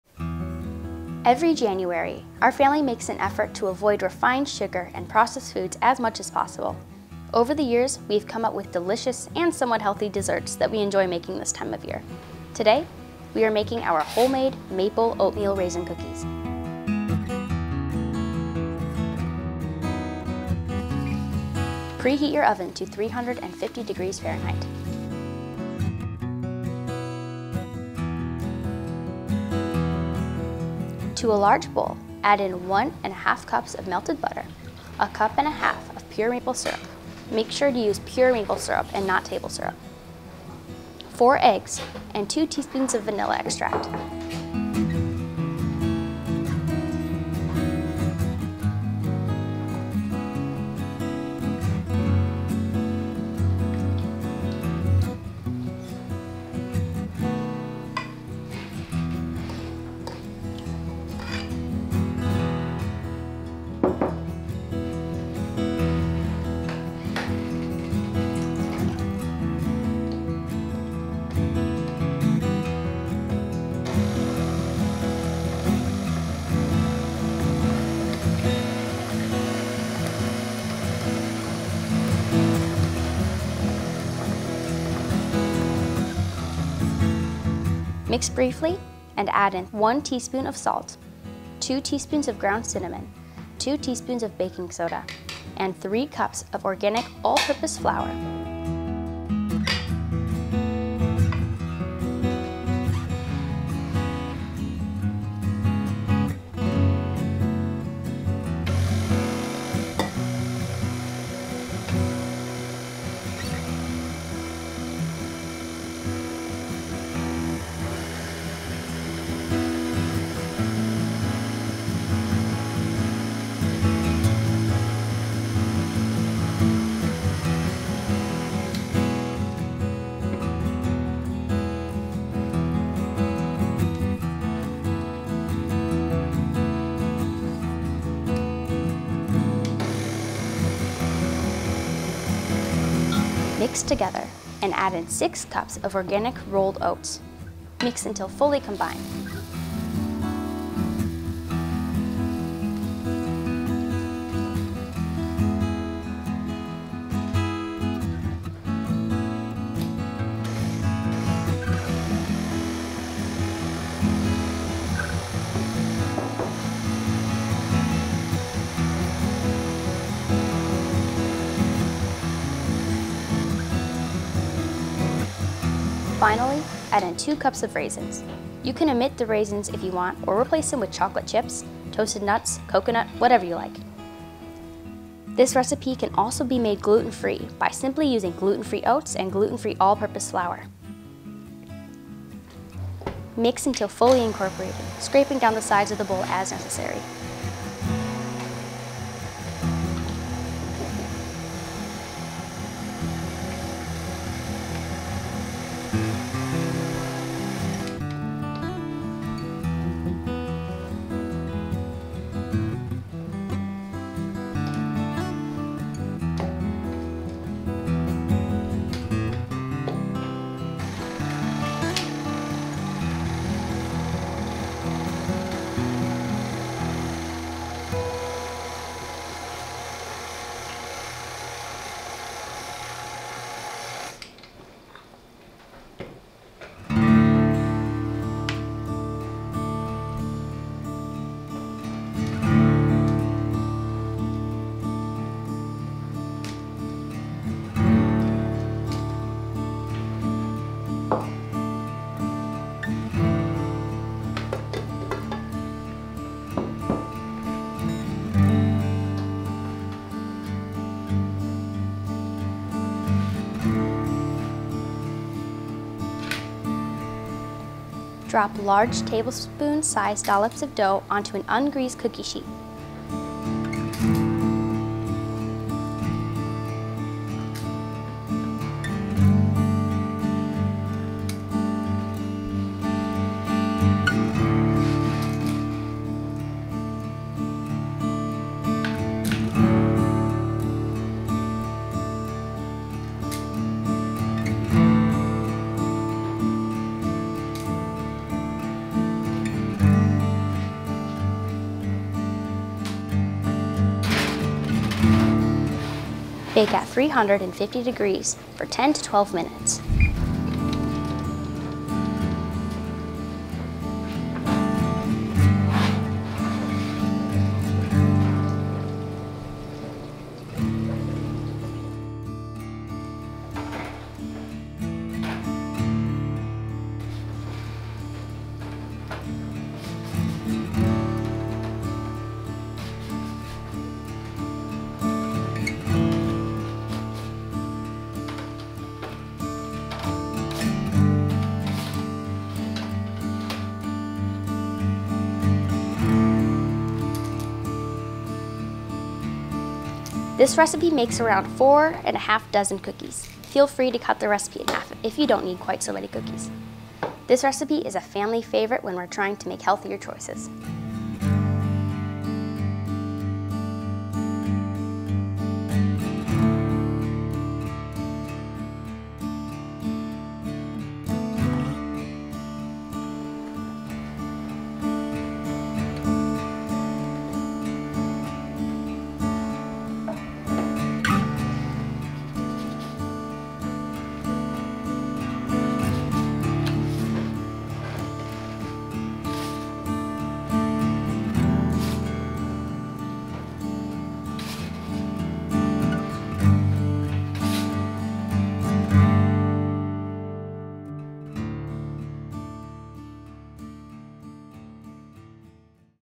Maple Oatmeal Raisin Cookies A Sugar Free and Gluten Free Cookie Recipe! | Healthy Cookie Recipes
We love recipes that are delicious and healthy at the same time, and these oatmeal raisin cookies with maple syrup sweetener are just the thing for a tasty, hearty dessert. This recipe can easily be altered to make it a delicious gluten free and refined sugar free option.

Baking tools as seen in the video:

Here's the recipe:
1 1/2 cups melted butter
1 1/2 cups pure maple syrup
4 eggs
2 teaspoons vanilla extract
3 cups all purpose flour (or GF all purpose flour)
1 teaspoon salt
2 teaspoons ground cinnamon
2 teaspoons baking soda
6 cups rolled oats (or GF oats)
2 cups raisins

Combine all ingredients in bowl. Drop dough balls onto baking sheet.
Bake at 350°f for 10-12 minutes. Makes around 4 1/2 dozen cookies. Enjoy!